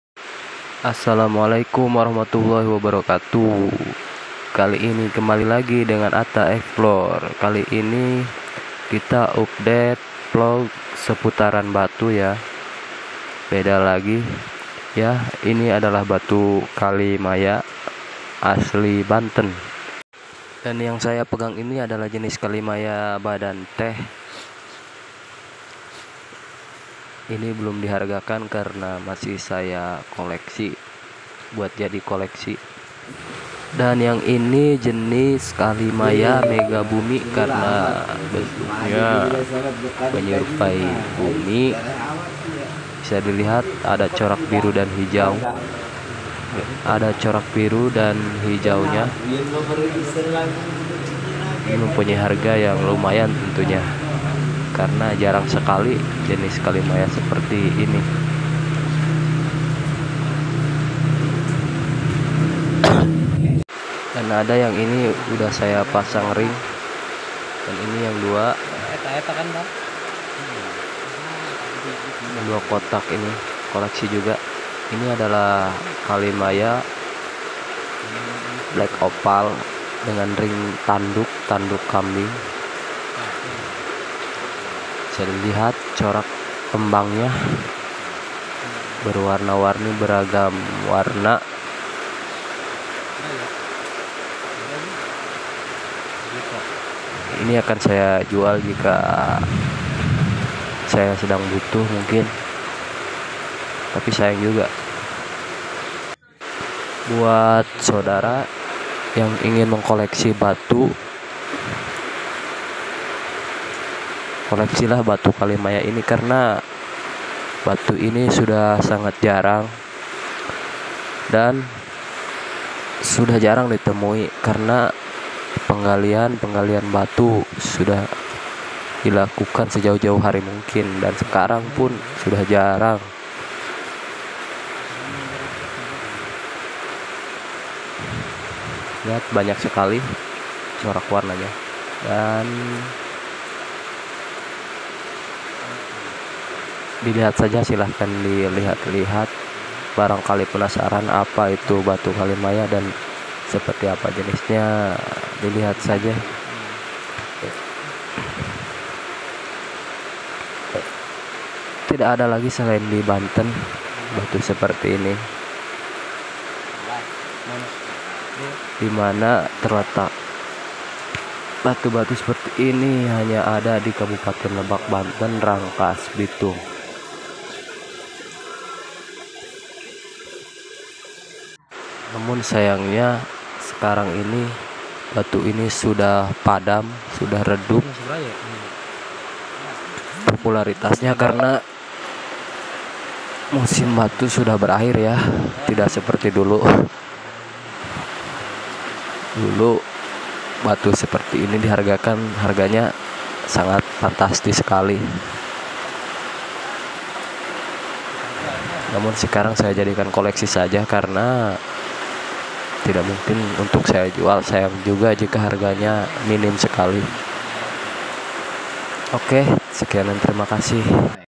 rare stones only exist in this area||batu kalimaya Banten asli
review batu kalimaya dengan corak beragam warna